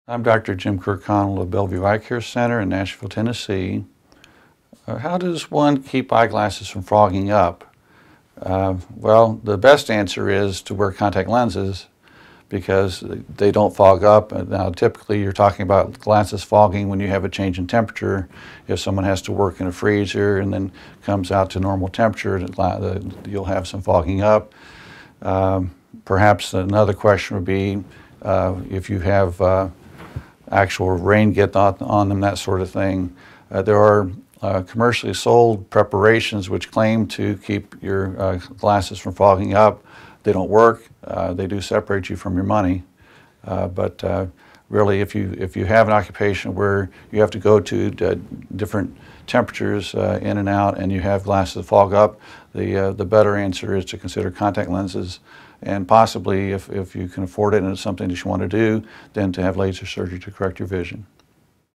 Eye Health & Optometry : How to Prevent Eyeglasses From Fogging Up
Products that claim to prevent eyeglasses from fogging up are often not effective, but wearing contact lenses instead of eyeglasses is one method to avoid the problem. Compare eyeglasses and contact lenses with information from a practicing optometrist in this free video on eye health.

Expert: James W. Kirkconnell
Bio: Dr. James W. Kirkconnell graduated from the University of Houston College of Optometry in 1984.